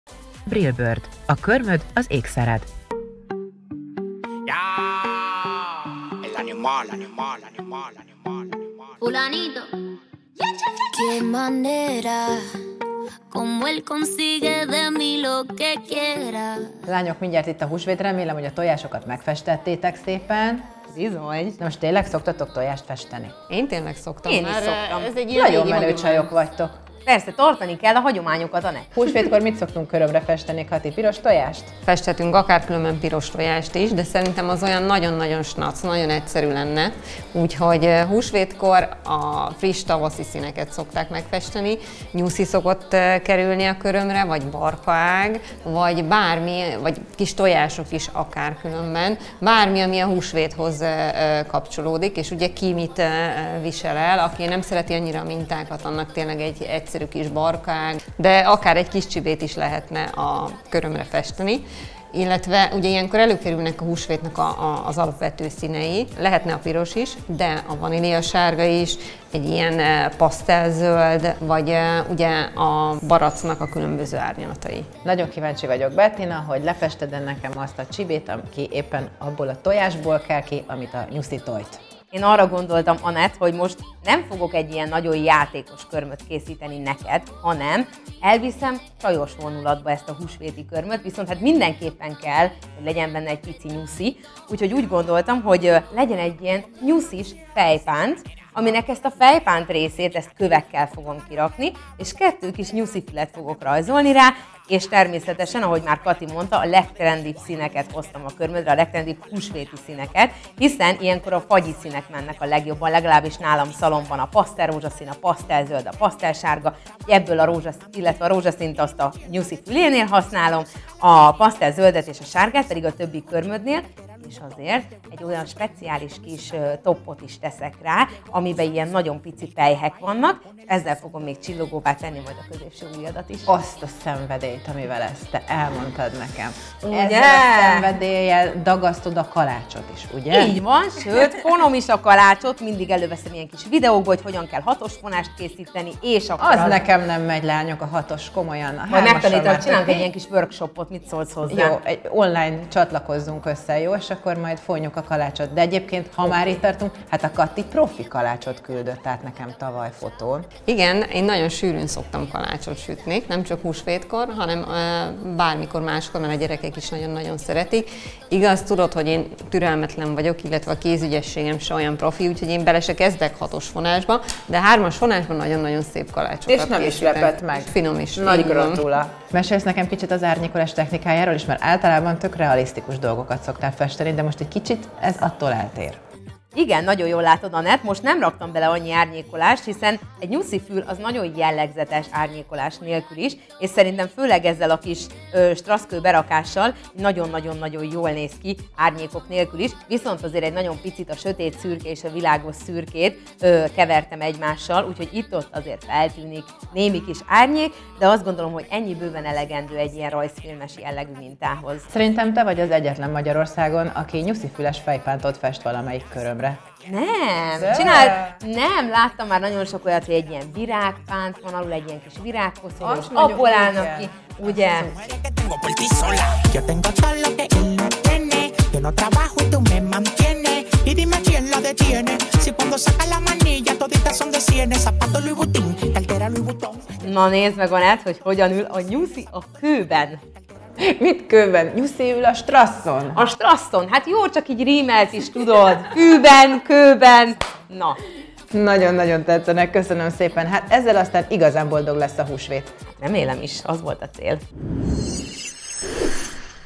Vaniliasárga csillogó nyuszifüles körmök a Trendmániában
Ahogy közeledik a tavasz egyre többen választanak pasztell színeket a körmeiken, most is egy gyönyörű vaniliasárgára esett a választásunk, a 106-os Hypnotic gél lakkra. Ne feledd már szín szerint is tudsz keresgélni webáruházban, itt